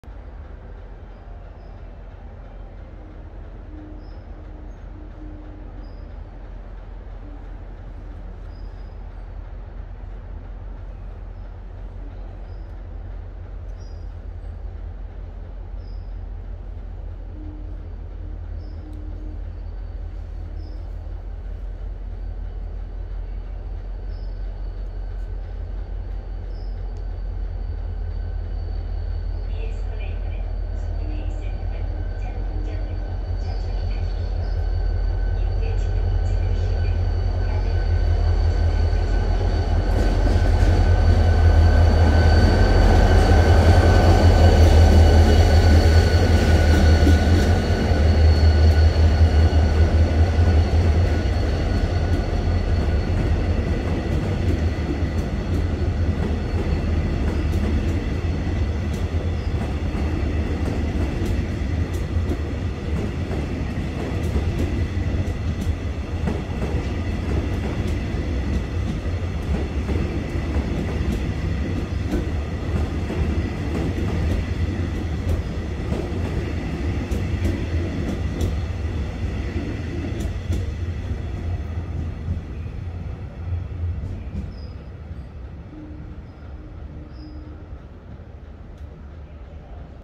[4K 60fps] 20230211 215210 갑종회송 5252 김천발 청량리행 서울4호선 서교공 다원시스 신차 4X51편성 상봉역 통과
촬영 : Samsung Galaxy Note 20 Ultra (SM-N986N)